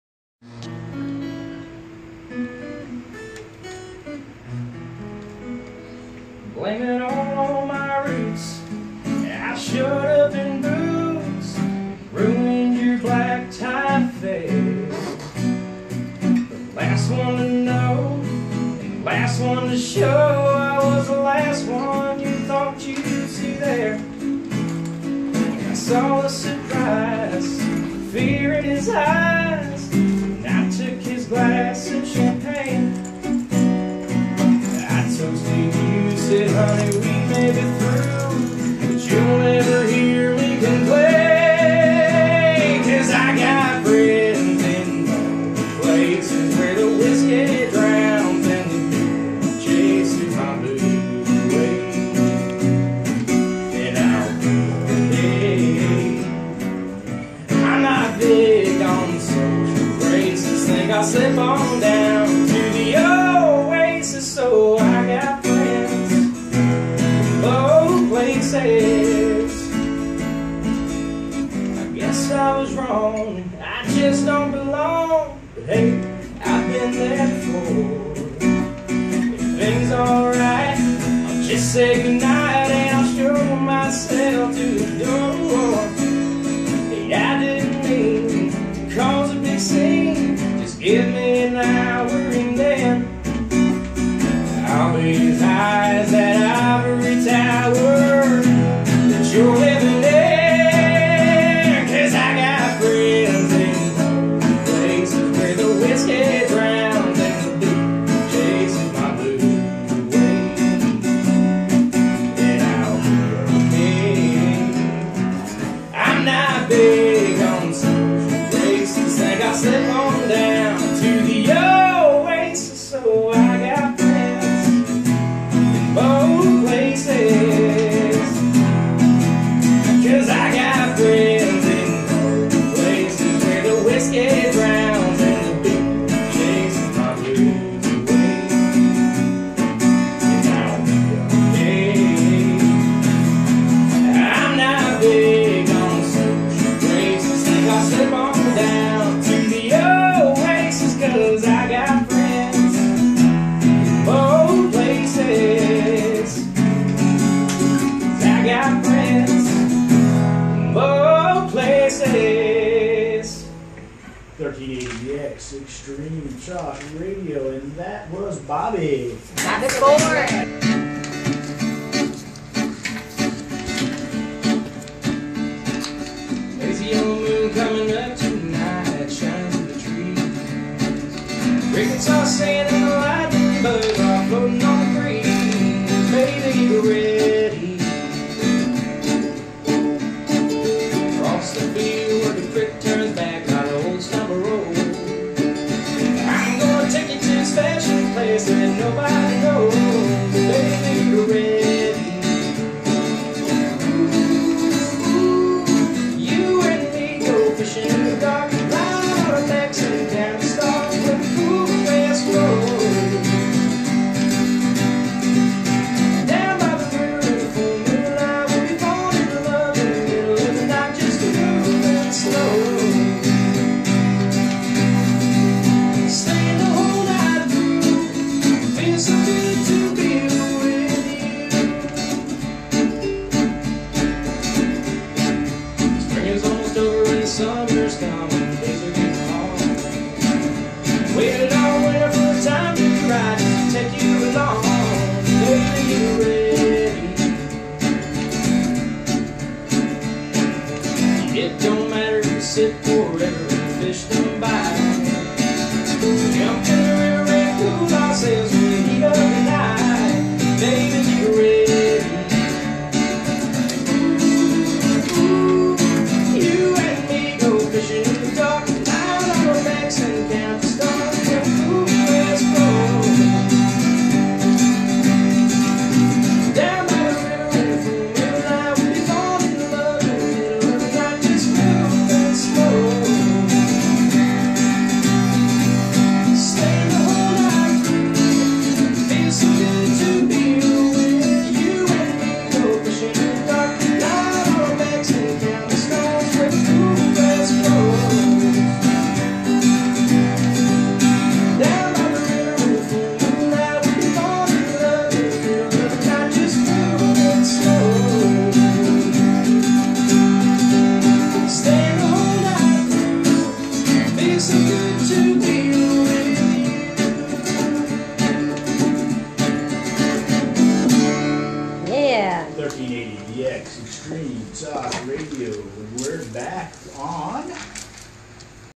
Bobby Ford on 1380 the X AM radio spot
Me preforming 2 songs on 1380 the X AM St. Louis

Vine: Bobby Ford